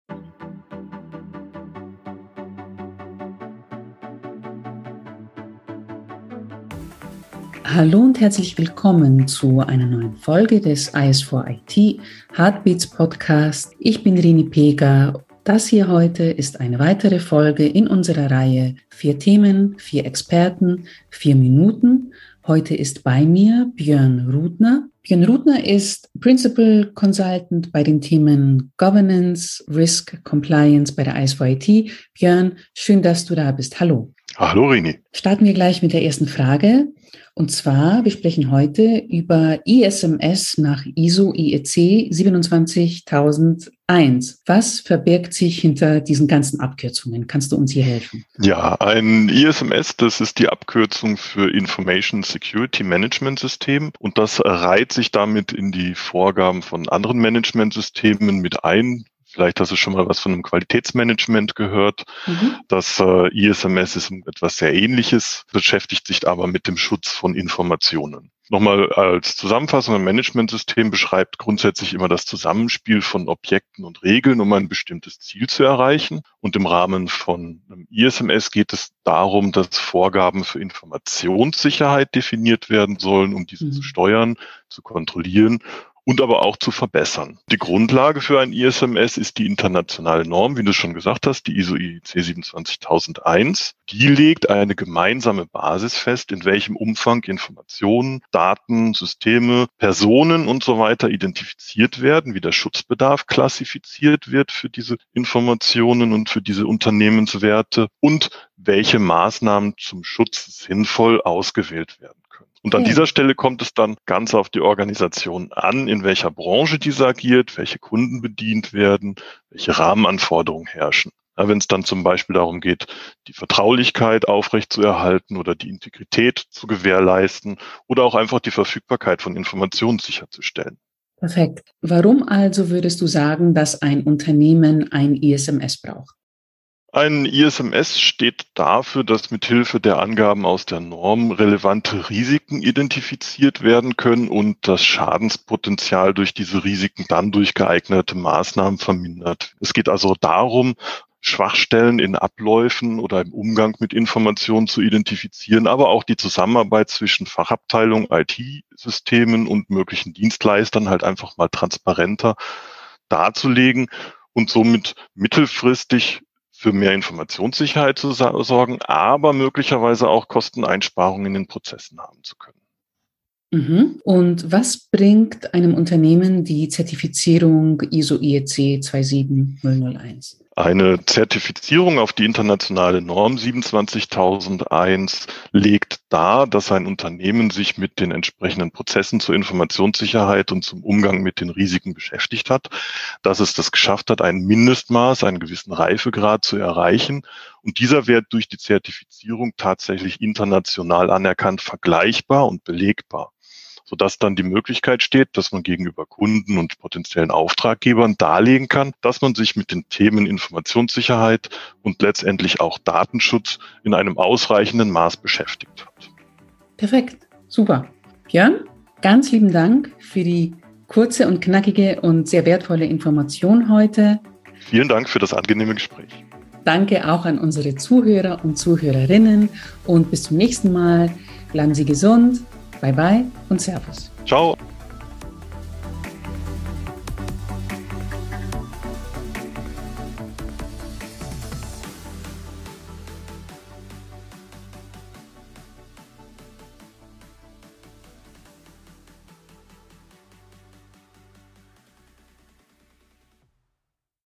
🔒 Was verbirgt sich hinter ISMS?: 4 Themen - 4 Experten - 4 Minuten
👉🏼 Nach Redteaming und Blueteaming mit SOC geht es diesmal um ISMS. Information Security Management-Systeme werden für mehr und mehr Unternehmen zur Pflicht. Die Vorteile sind weniger bekannt.

🎙️ In der dritten Folge unserer Podcast-Reihe "4 Themen - 4 Experten - 4 Minuten" gibt uns Björn Rudner einen ersten Überblick über den Nutzen eines ISMS und die in den meisten Fällen damit verbundene Zertifizierung nach ISO/IEC 27001.